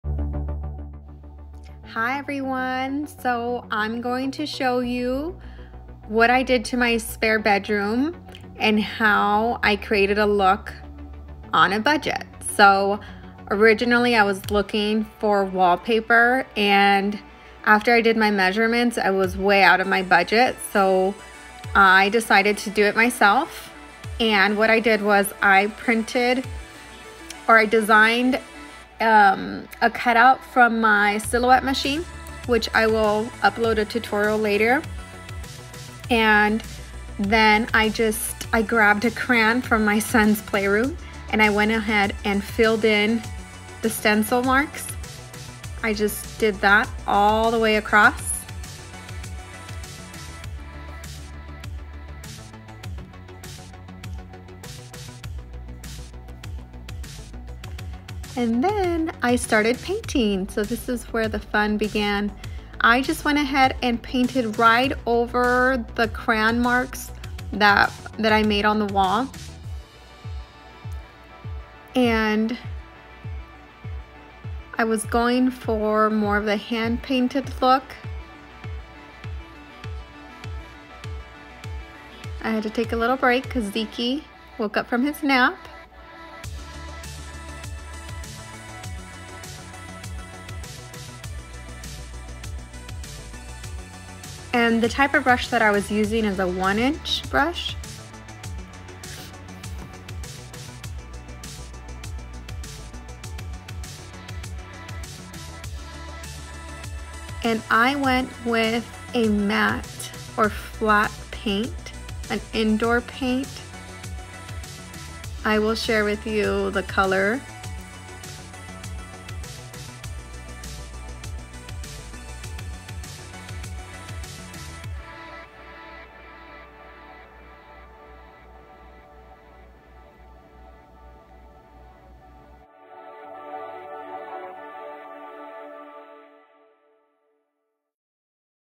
DIY Hand Painted Accent Wall
How to get a wallpaper look on a budget using a stencil, crayon, and paint!